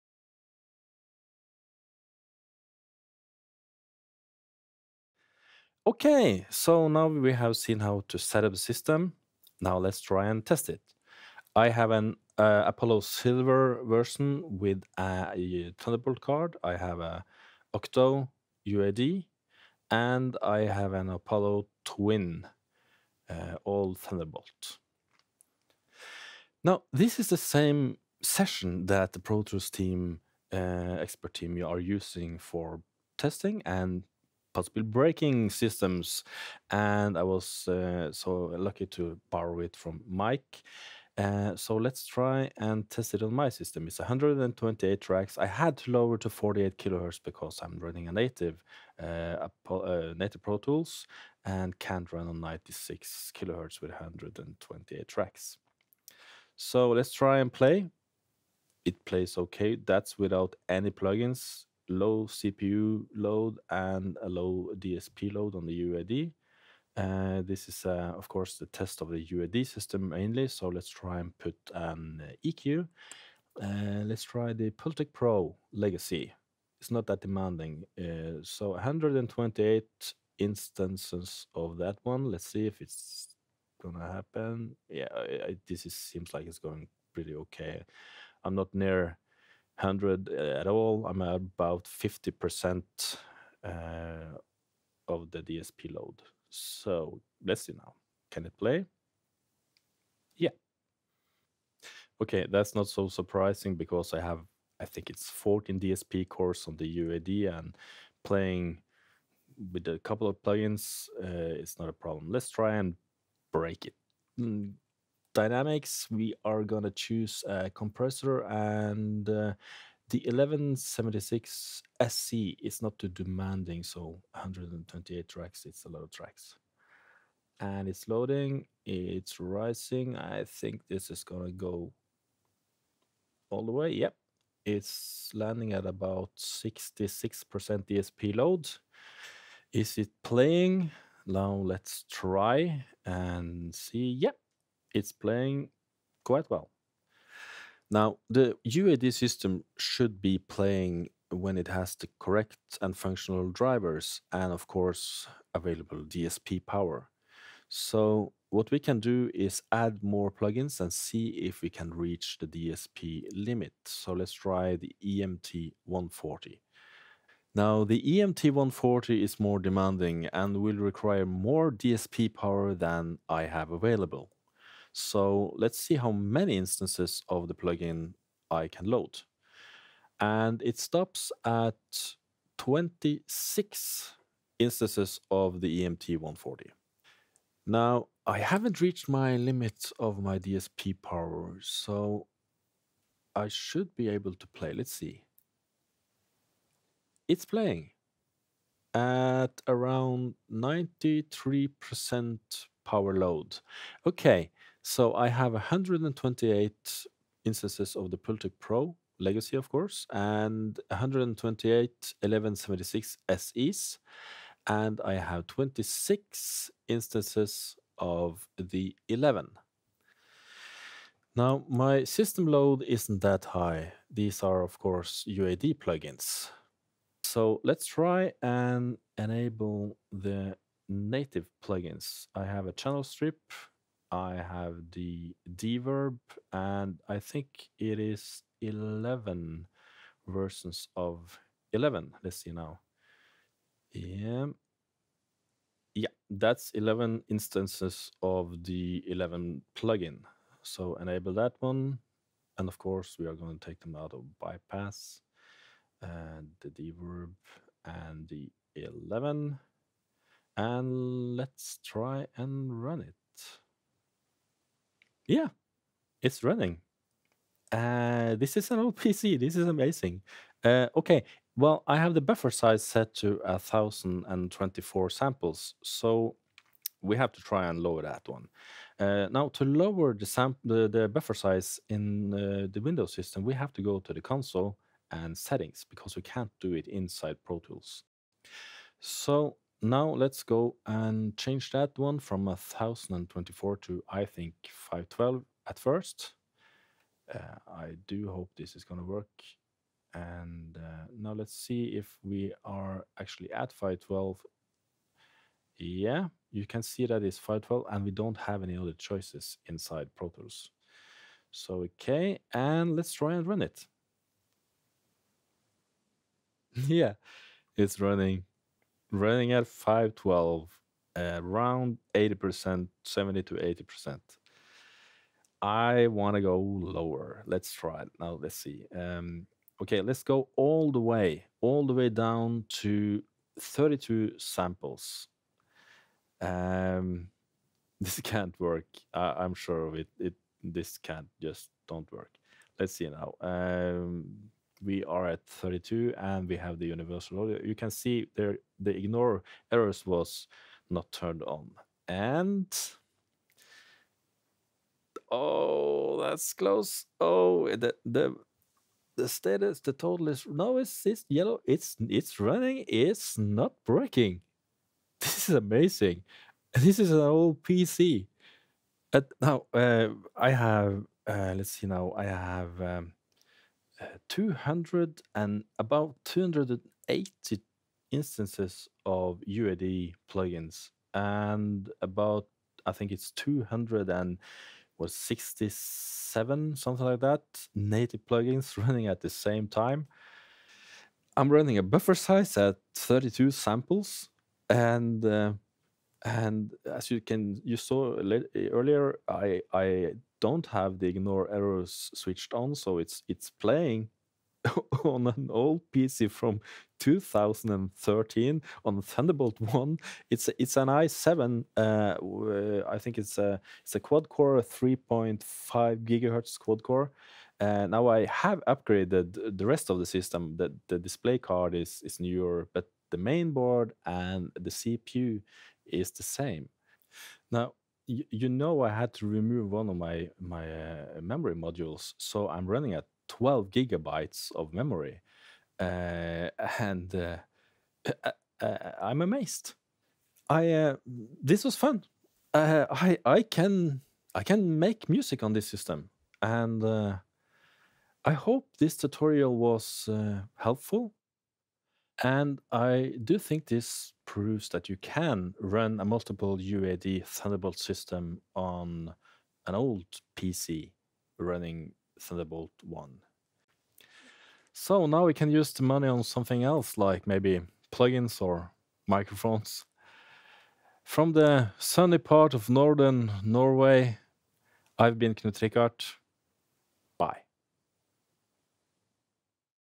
Universal Audio 9.2 multi thunderbolt system running on a Windows 10 PC with thunderbolt 1
This video is part of a tutorial on Pro Tools Expert on getting an Universal Audio Apollo Multi Thunderbolt system working on an old unsupported Windows PC running Windows 10 and Thunderbolt 1.